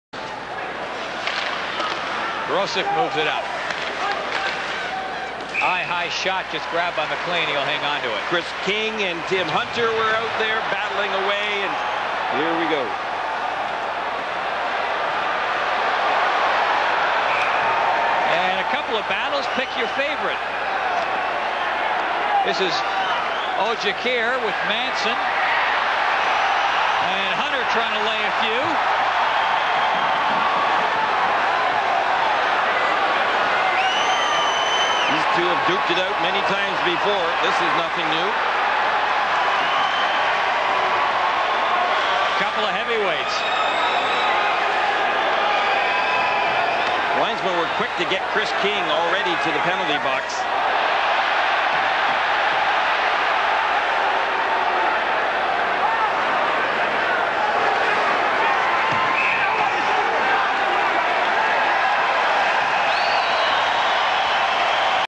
Dave Manson vs. Tim Hunter (2-9-95)
Dave Manson of the Winnipeg Jets gets into a tussle with Tim Hunter of the Vancouver Canucks.